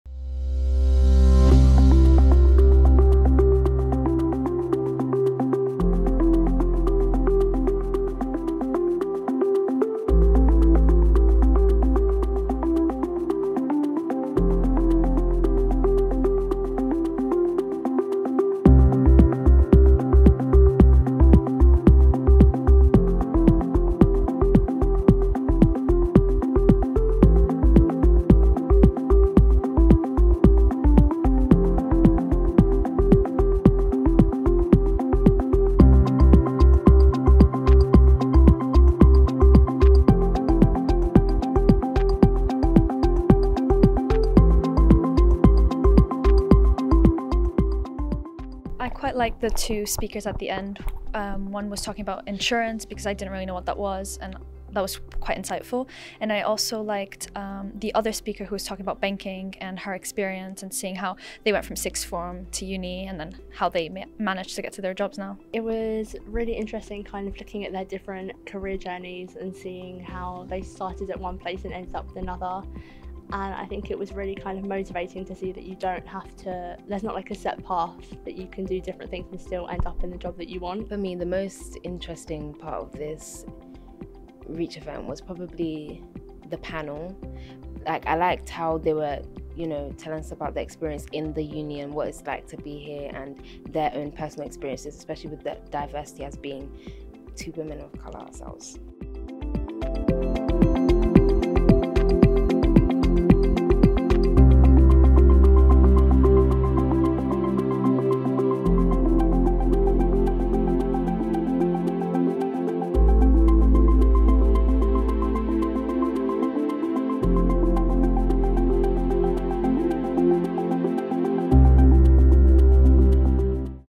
This is your sign to attend our next REACH event
Thinking of a career in banking and finance but not sure if it’s for you?

Our REACH events focus on championing diversity in the industry and supporting underrepresented groups to achieve success in the financial sector.

These events hear from expert panels, key industry leaders and LIBF professionals on what it means to be from a diverse background, and still have a successful career.